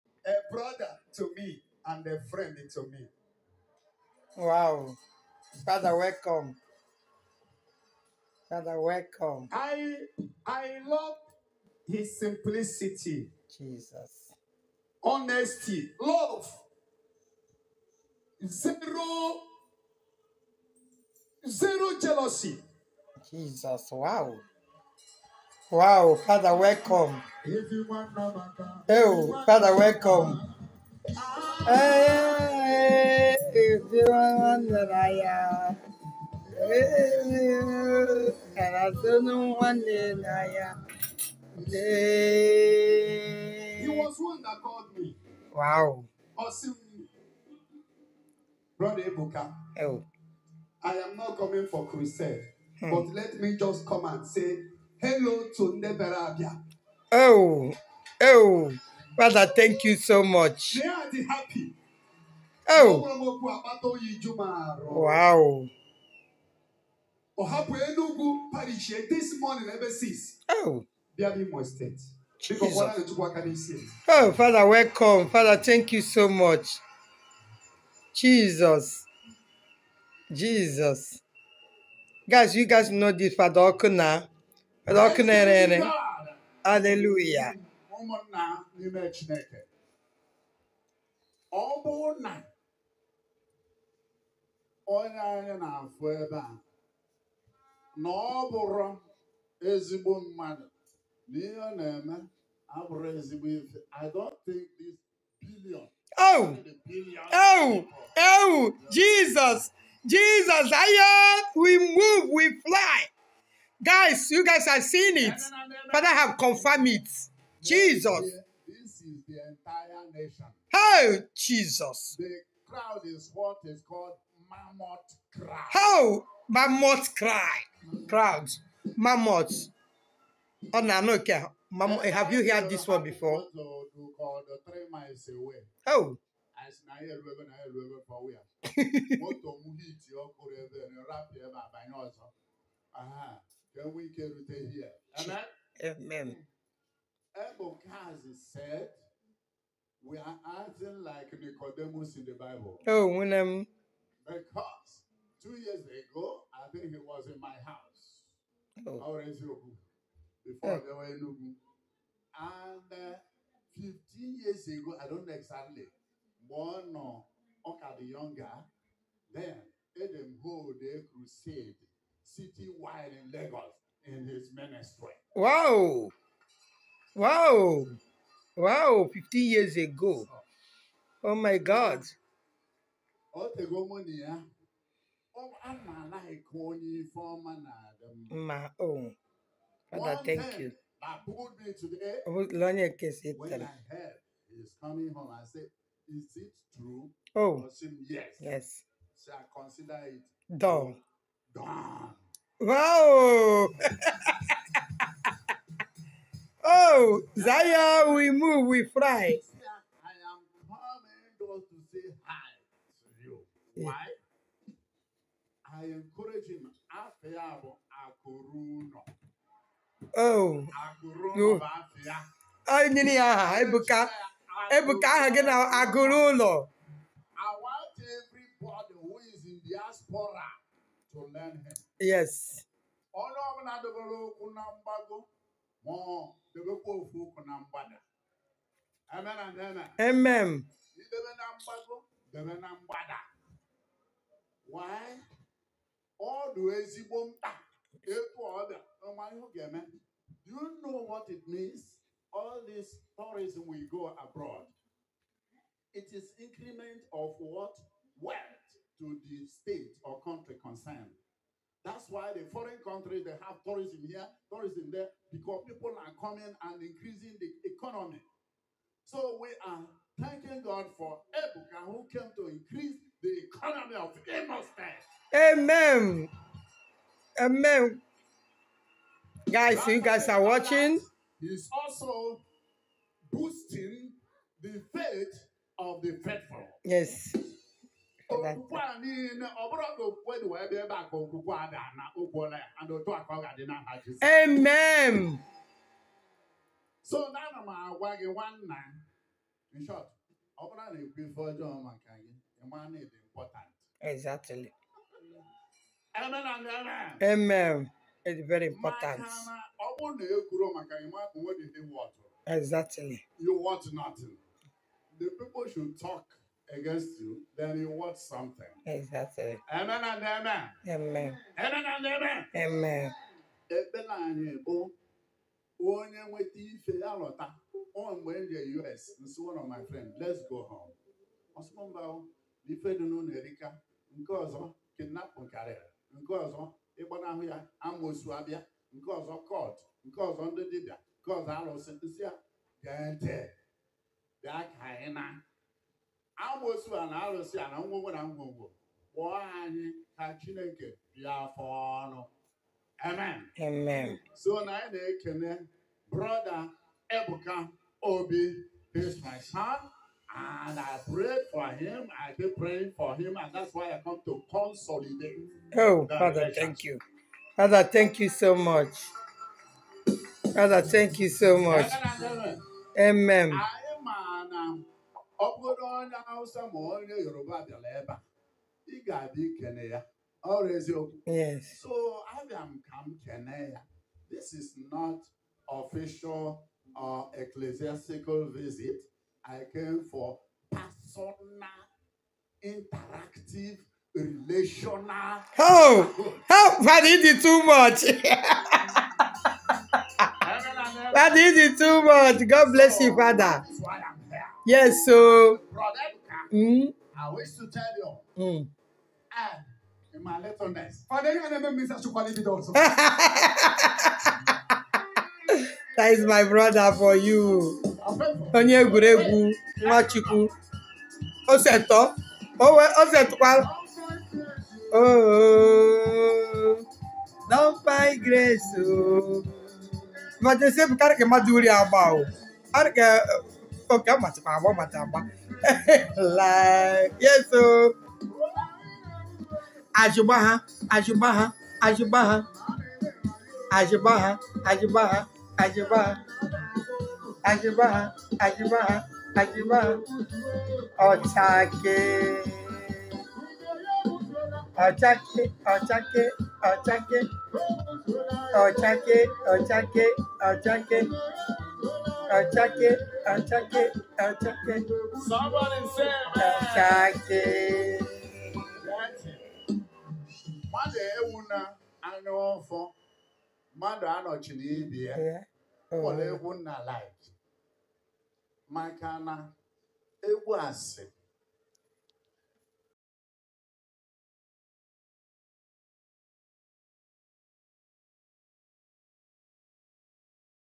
Father Oku Nerere Honors Zion Ngor Okpala at Crusade Day 2
REV.FATHER OKU NERERE PAY HOMAGE TO ZION NGOR OKPALA  DAY 2 OF 3 DAYS CRUSADE #2024

EVANG EBUKA OBI WELCOME REV.FATHER OKU NERERE AS HE PAY HOMAGE TO ZION CRUSADE NGOR OKPALA 2024